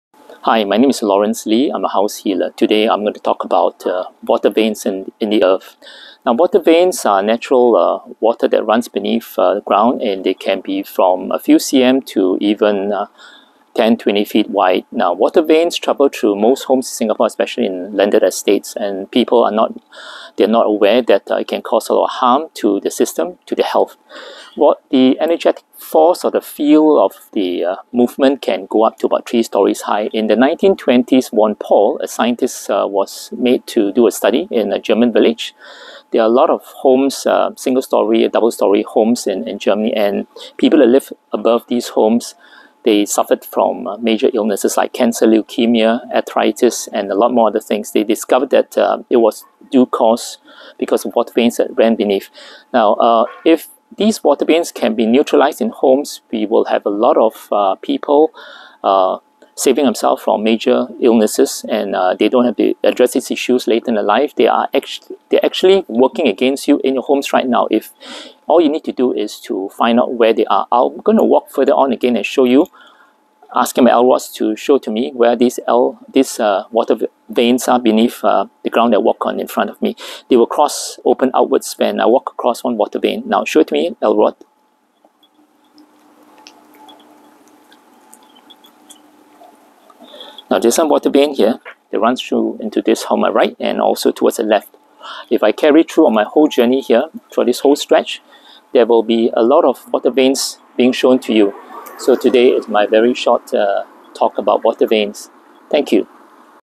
House Healing (Dowsing): Running Water Beneath Your Home Can Make You Sick (Geopathic Stress)
Running Water Beneath Your Home Can Make You Sick (Geopathic Stress).

The earth that we walk on is a makeup of different soil and rock formations, underground streams, fractures, and mineral stones. It is a living entity and it radiates geomagnetic and electromagnetic fields (EMF). Water Veins/Underground Water have memory, recording information of thoughts, actions and emotions of people above it as it travels underneath the land. Water responds to human emotions as Masaru Emoto’s research has shown. As these streams of water that carry EMF travel beneath homes, they also absorb people's vibration of pain, suffering, anxiety and illness. Causing disharmony to others that live along that length of the water vein. Affecting one’s physical, emotional, mental and spirit state. Resulting in headaches, memory loss, insomnia, arthritis, skin rashes, pain in nerve pathways, nose bleeds, cancer and more.

The science behind RF (radio frequencies) and pulsed microwave radiation commonly known as EMF(electromagnetic fields) has been taken too lightly for some time. Prolonged exposure to these radiations and the earth’s natural negative frequencies can affect the immune system and cause serious health problems (Baron Gustav Freiherr Von Pohl, Earth Currents: Causative Factor of Cancer and Other Diseases, Frech-Verlag 1988).